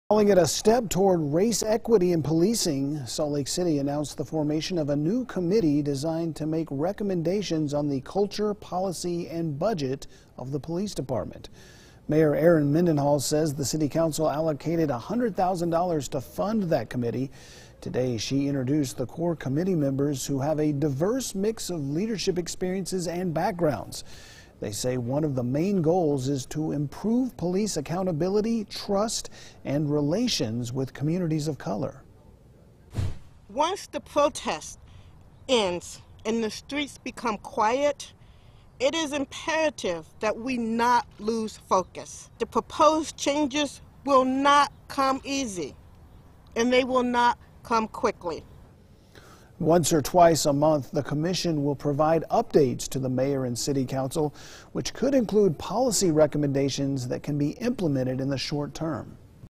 Salt Lake City leaders announce Commission on Racial Equity in Policing (4 p.m.)
Salt Lake City leaders announced the implementation of a new advising council that will make recommendations to the Mayor and City Council on Salt Lake City’s policy, budget, and culture of policing.